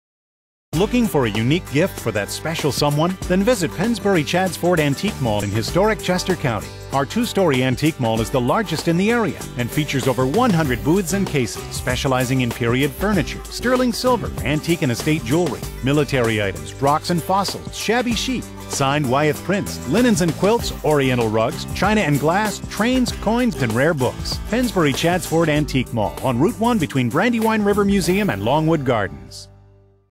Pennsbury Chadds-Ford Antique Mall
The Pennsbury Chadds-Ford Antique Mall has  been offering Quality Antiques & Collectibles for over 36 years.  The Air-conditioned  2-Story building has over 100 dealers and 180 booths & display cases.
PA's Pennsbury Antique Mall is located on route 1 in historic Chadds-Ford, PA.  Chadds Ford is in Chester County, approximately 20 miles south-west of Philadelphia, Pennsylvania and the area is known for its rich history.  Other local Atractions are Longwood Gardens, the Brandywine Battlefield, the Brandywine River Museum, the Delaware Museum of Natural History, Winterthur Museum, and the Chadds-Ford Winery.